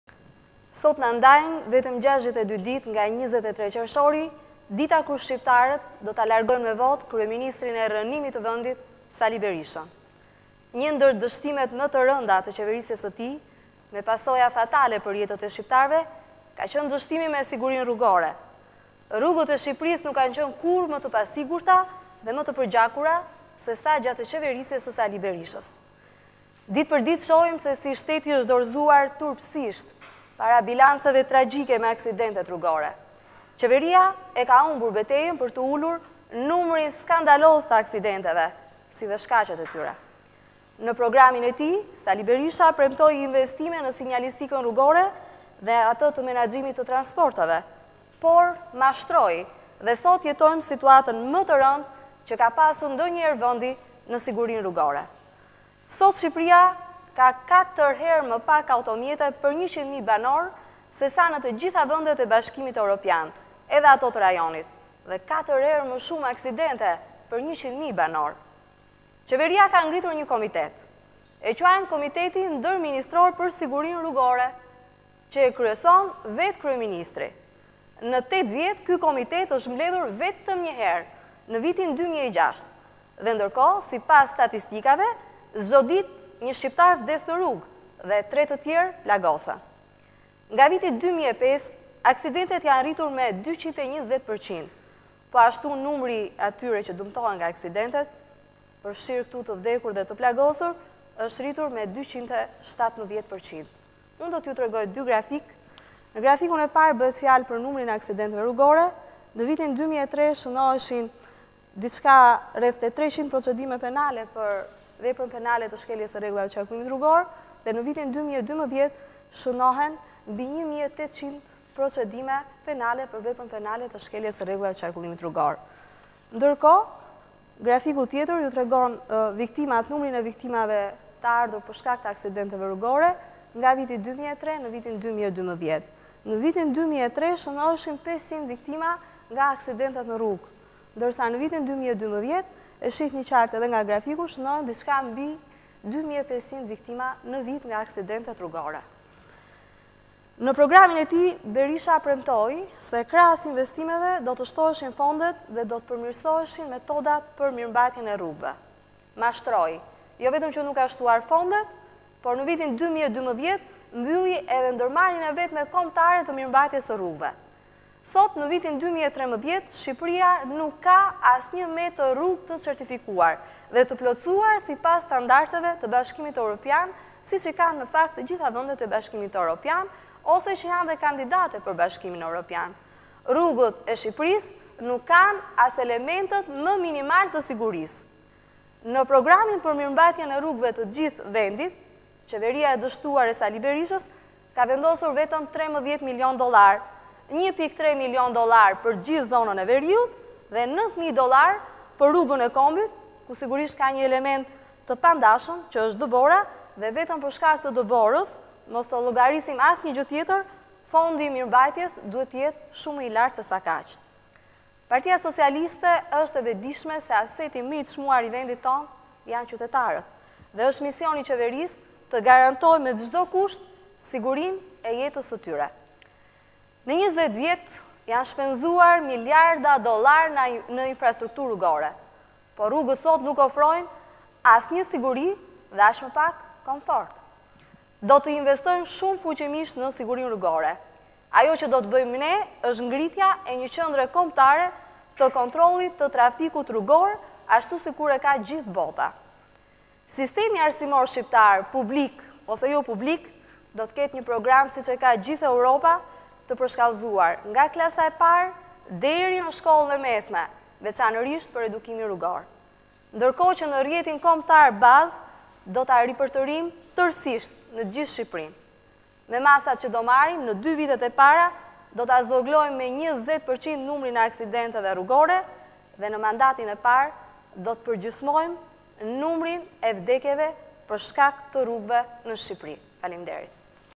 Aksidentet në rritje, asnjë metër rrugë me standarde BE, dështimi i radhës i Saliut
Deklaratë për media e deputetes Ermonela Felaj.